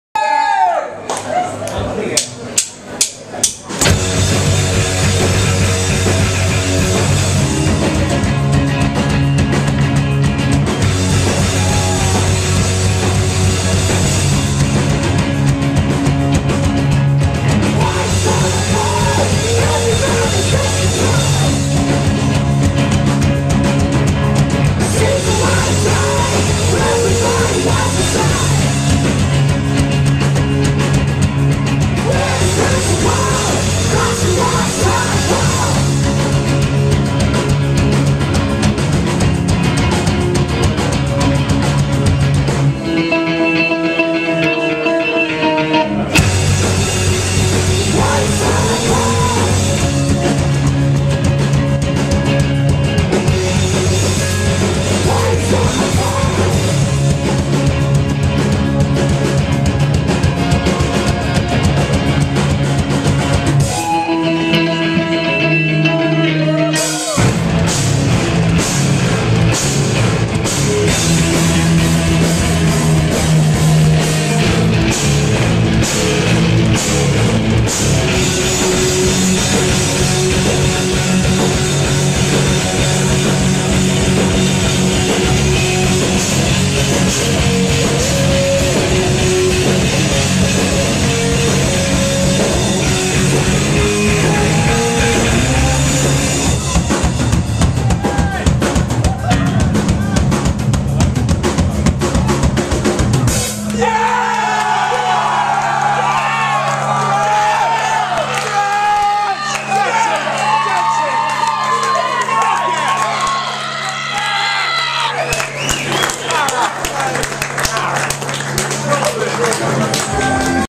CODEX LEICESTER @ Firebug, Leicester, UK.
White Noise Charity Festival - Celebrating Leicester's home grown talent